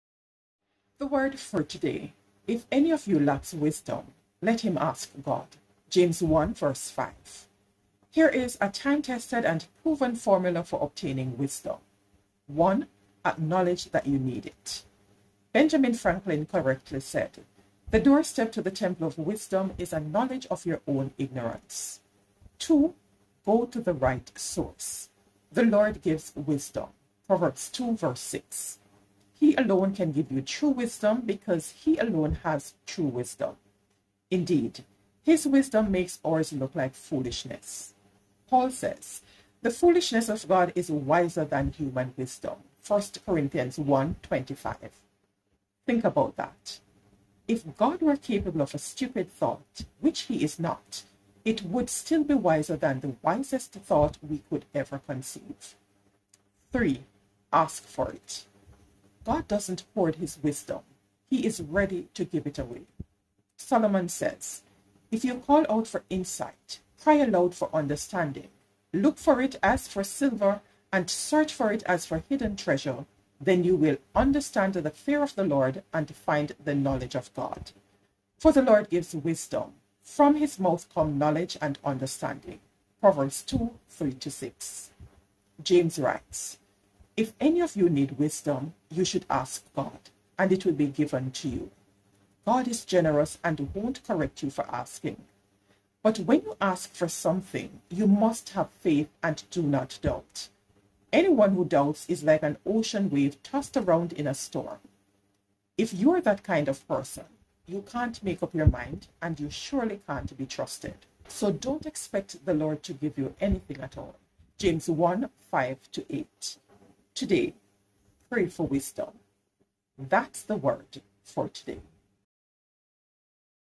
Devotional | PRAY FOR WISDOM (4)
'If any of you lacks wisdom, let him ask of God.' James 1:5 NKJV

Here is a time-tested and proven formula for obtaining wisdom: (1) Acknowledge that you need it. Benjamin Franklin correctly said, 'The doorstep to the temple of wisdom is a knowledge of your own ignorance.' (2) Go to the right source. 'The Lord gives wisdom' (Proverbs 2:6 NKJV). He alone can give you true wisdom because he alone has true wisdom. Indeed, his wisdom makes ours look like foolishness. Paul says, 'The foolishness of God is wiser than human wisdom' (1 Corinthians 1:25 NIV). Think about that. If God were capable of a stupid thought, which he is not, it would still be wiser than the wisest thought we could ever conceive. (3) Ask for it. God doesn't hoard his wisdom; he is ready to give it away. Solomon says: 'If you call out for insight...cry aloud for understanding...look for it as for silver and search for it as for hidden treasure, then you will understand the fear of the Lord and find the knowledge of God. For the Lord gives wisdom; from his mouth come knowledge and understanding' (Proverbs 2:3-6 NIV).

James writes: 'If any of you need wisdom, you should ask God, and it will be given to you. God is generous and won't correct you for asking. But when you ask for something, you must have faith and not doubt. Anyone who doubts is like an ocean wave tossed around in a storm. If you are that kind of person, you can't make up your mind, and you surely can't be trusted. So don't expect the Lord to give you anything at all' (James 1:5-8 CEV). Today, pray for wisdom.